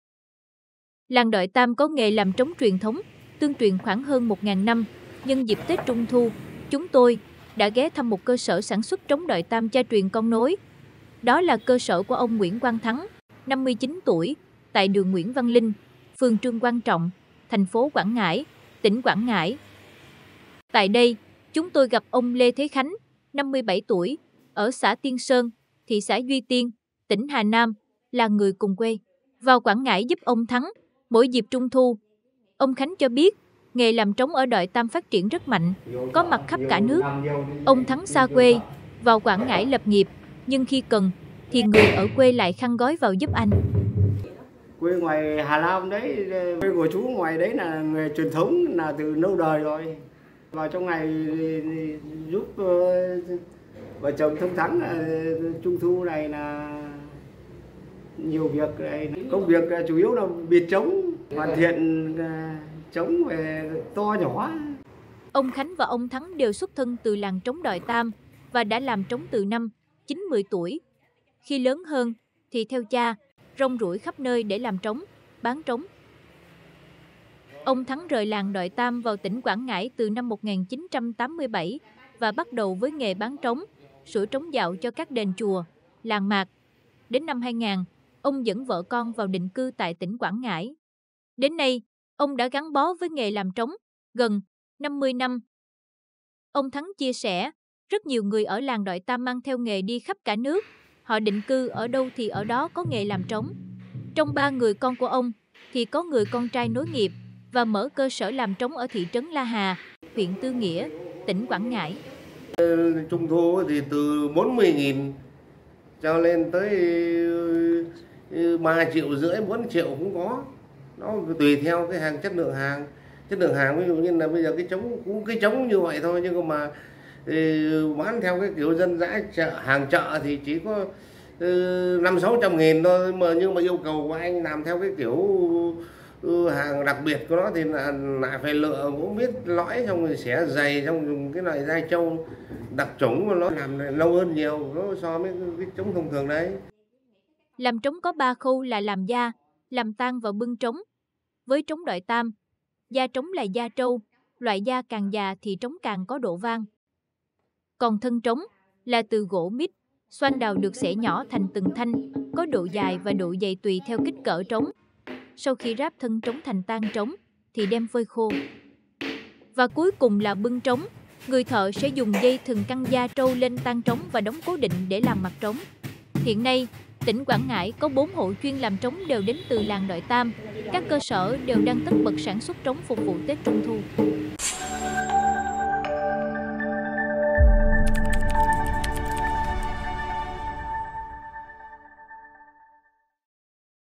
Làm trống Đọi Tam ở xứ Quảng dịp Tết Trung thu
Làng Đọi Tam có nghề làm trống truyền thống, tương truyền khoảng hơn 1.000 năm, nhân dịp Tết Trung thu, chúng tôi đã ghé thăm một cơ sở sản xuất trống Đọi Tam “cha truyền con nối”.